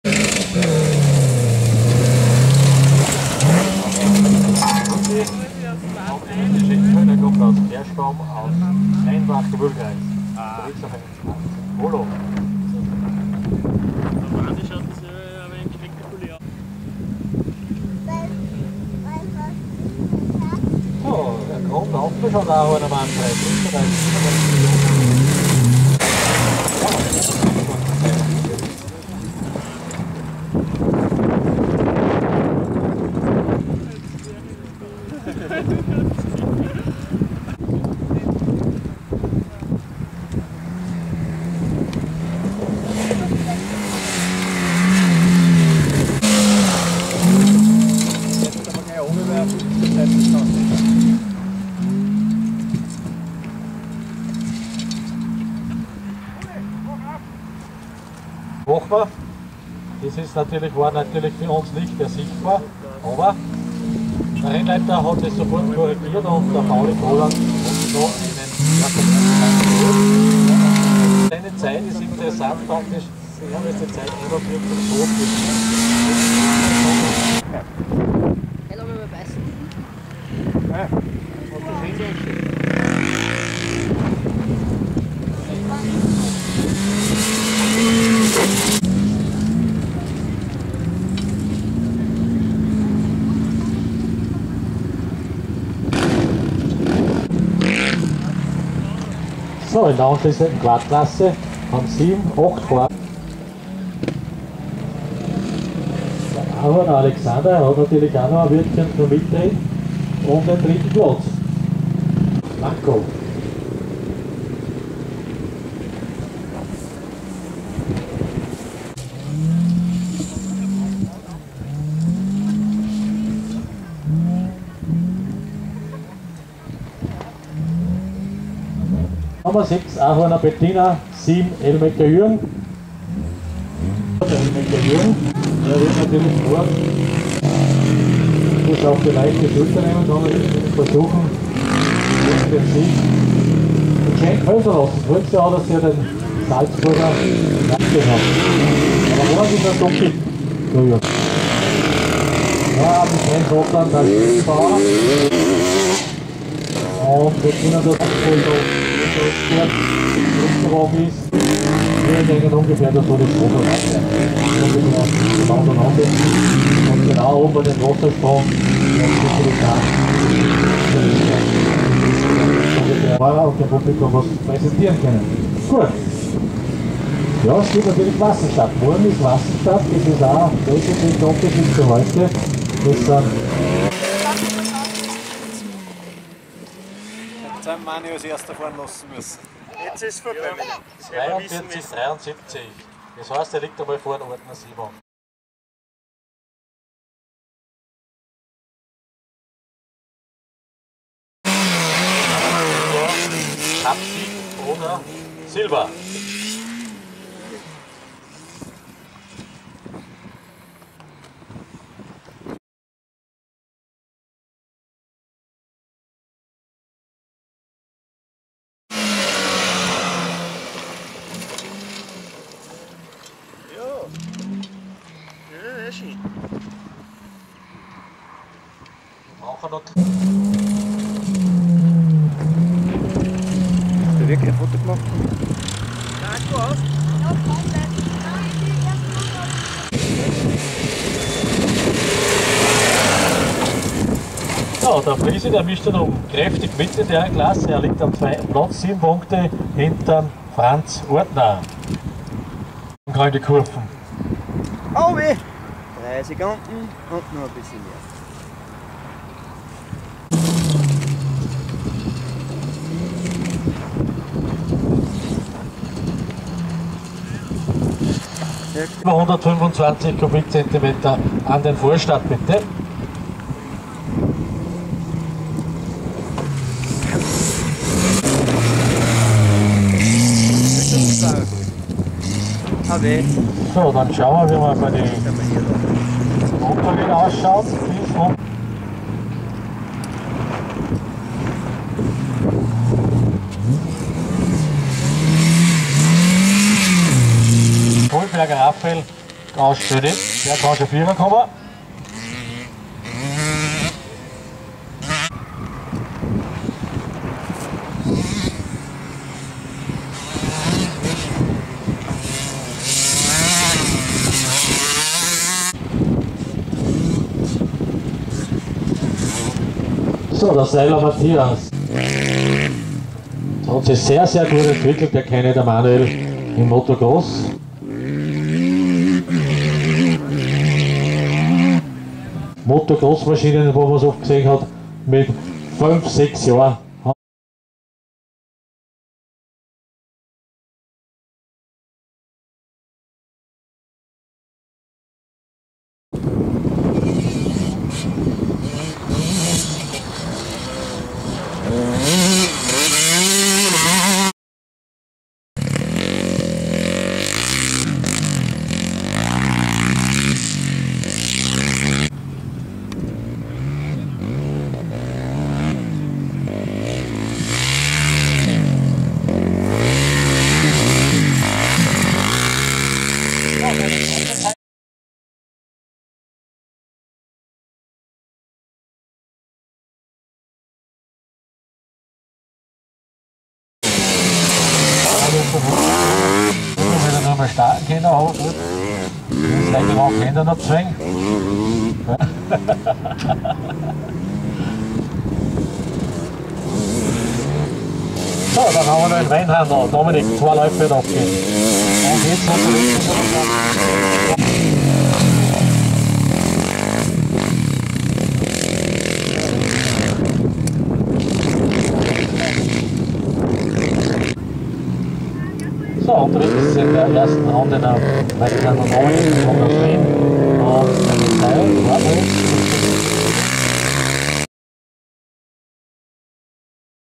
Mühlviertler Slalomcup Schenkenfelden, 06.10.2012 Teil 1 von 2
Letzter Lauf zum Mühlviertler Slalomcup am Samstag. Wetter war relativ schön, nur störend war der Wind. Zum Abschluss gab es noch schöne Heber. Video 1  PKW I, Motorräder und Quad.